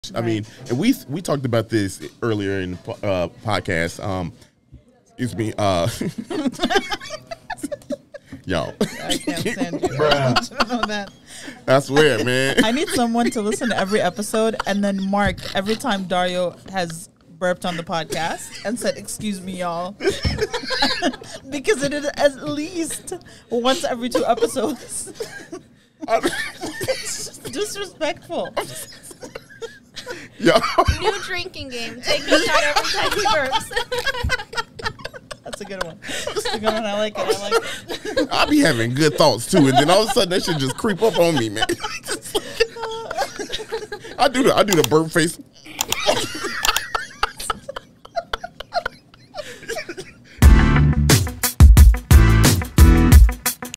Burpface
Y’all, please understand…

This week we are in SouthLawndale with Cristy of the @lvcfchicago !! We eat at an absolute gem La Chapparita Grocery! There’s a lot of fun and knowledge here! Trust us lol